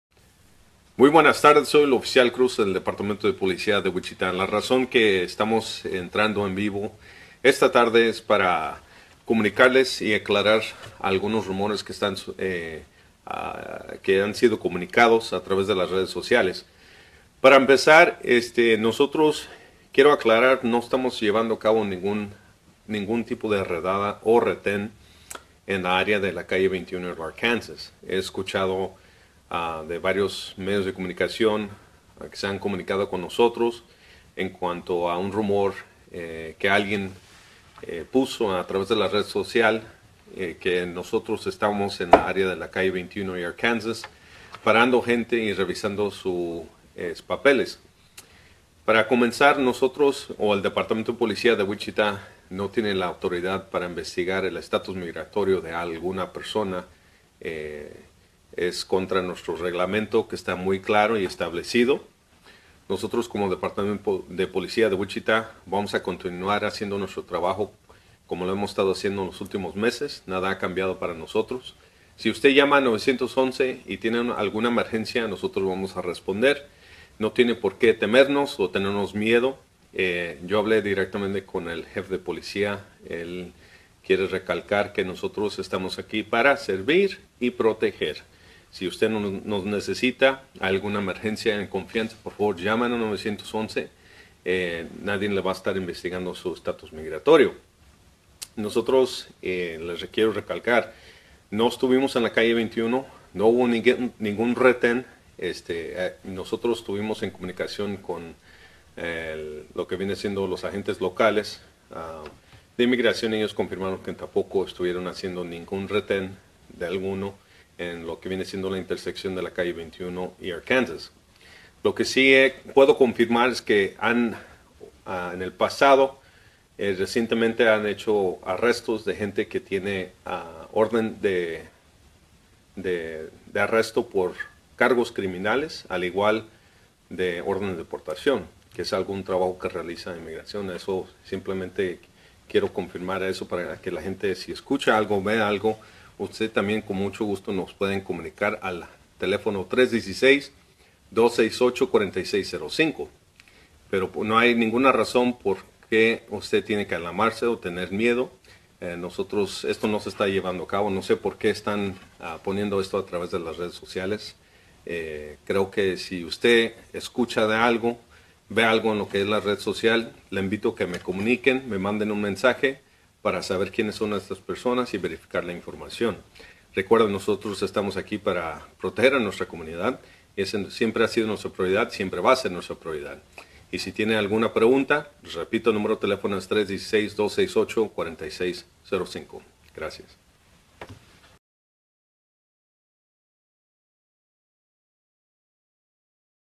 Officer Paul Cruz dispels a rumor about immigrants being pulled over indiscriminately
Officer Paul Cruz has been reaching out to the spanish-speaking community in Wichita. Last week he dispelled a rumor that immigrants were being randomly detained at 21st and Arkansas.